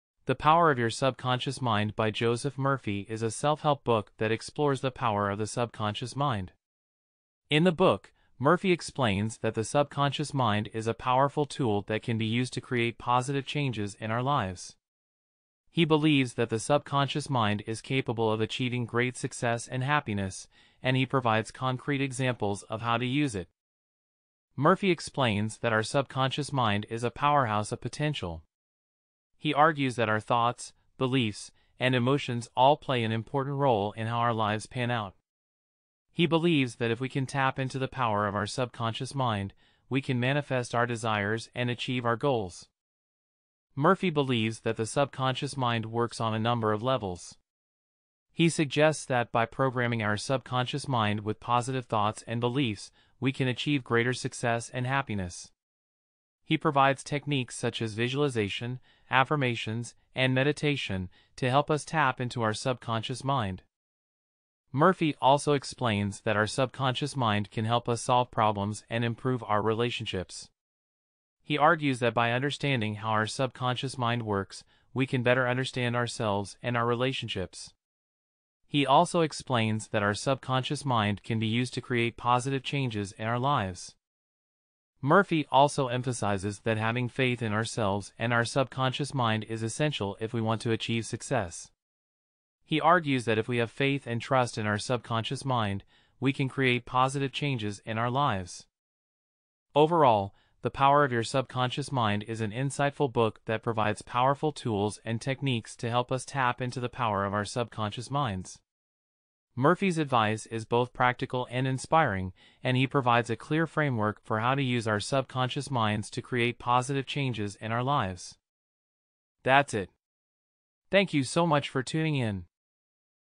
The Power of Your Subconscious Mind by Joseph Murphy 2 Minute Book Summary
I love coffee! This book is a classic self-help guide written by author Joseph Murphy, a religious leader and psychotherapist. It was first published in 1963, and since then has become a staple of self-help literature. In this book, Murphy explains how the power of the subconscious can help you achieve success, health, and well-being. He provides practical advice on how to access this power, and how to use it to create positive change in your life. By learning how to direct and control your subconscious mind, Murphy explains, you can unlock your full potential and create the life you desire. I hope this summary has been helpful!